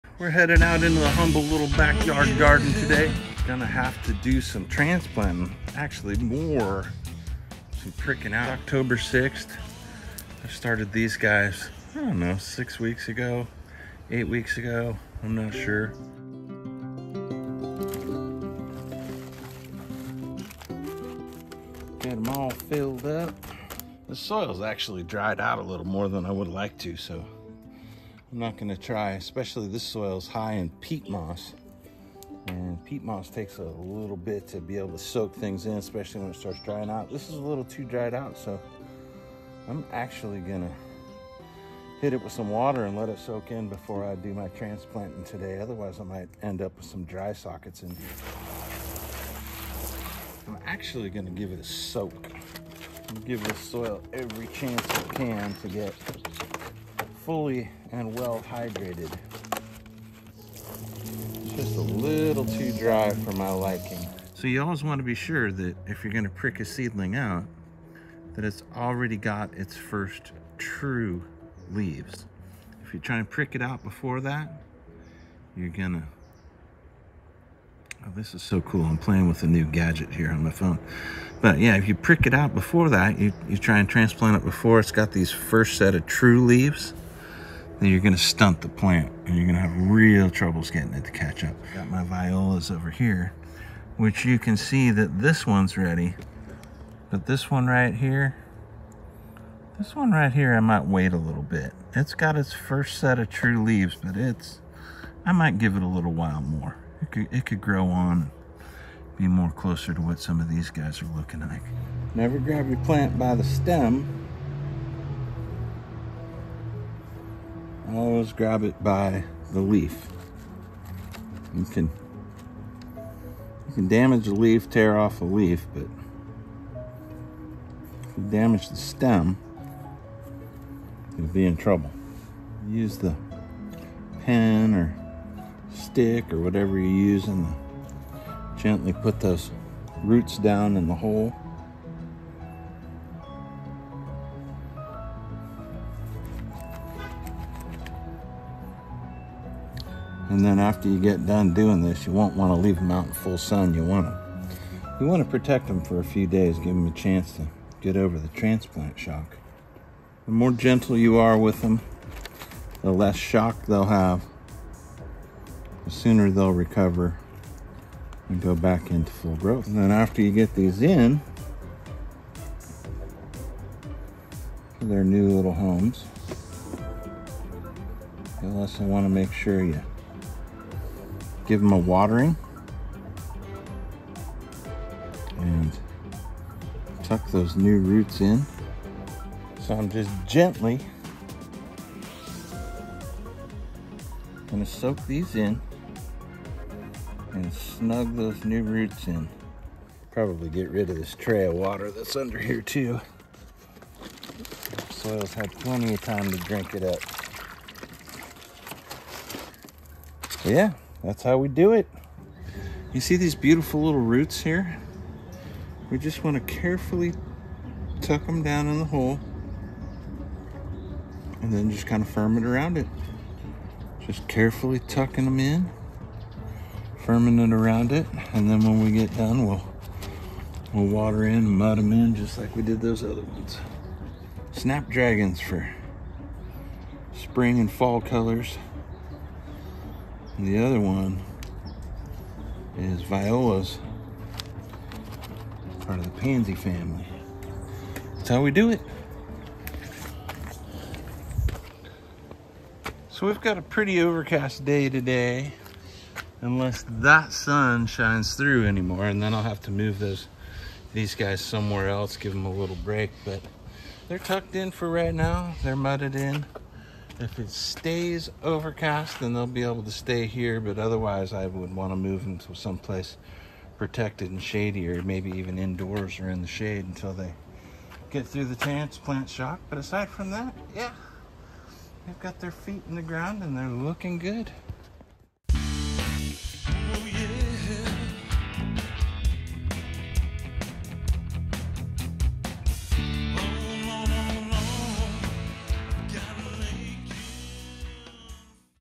Garden Zone 9b Transplanting Fall Flower Seedlings - Snapdragons and Viola's in October
Garden Zone 9b See how I prick out and transplant (flower seedlings) this fall for our early spring color.  Our Snapdragon's and Violas for Winter/Spring color in our flower garden, are ready to transplant here in Pleasant Hill California 94523. Garden Life- what's happening here in the Garden 94523 in October?

Thanks